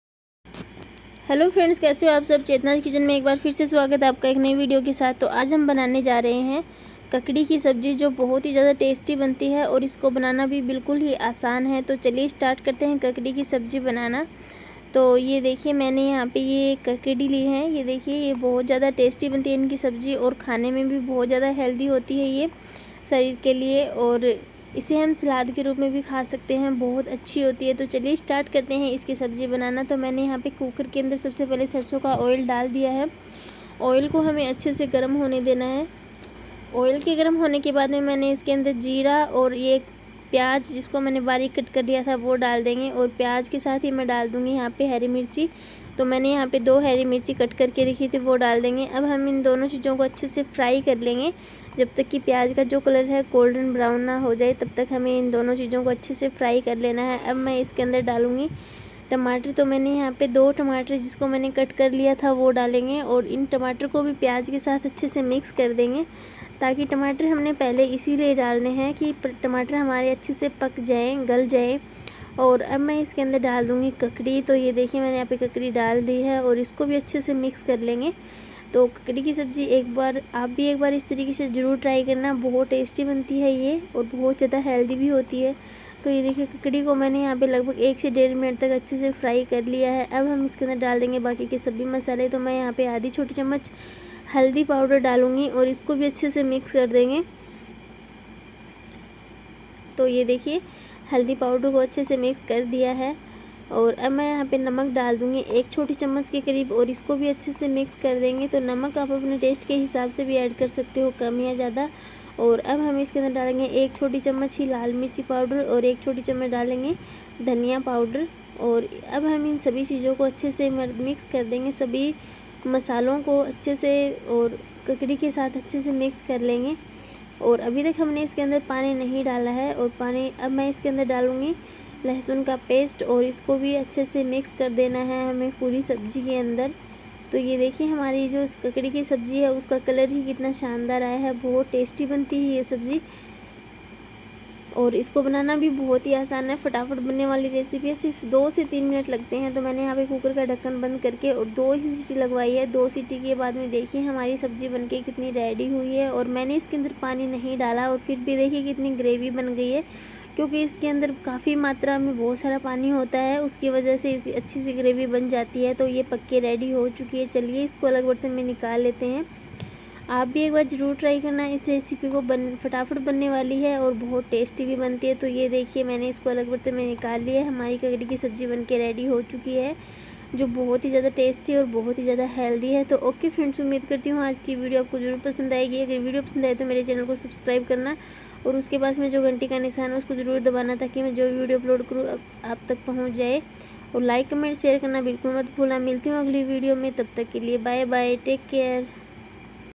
ककड़ी की सब्जी ऐसे बनाये जो स्वाद और सेहत दोनों से भरपूर हो //Kakdi ki sabji //kakdi ki recipe
Kakdirecipe, kakdi ki sabji, kadkdi ki sabji kaise banaye, kakdi ki sabji banane ki sabji asan vidhi,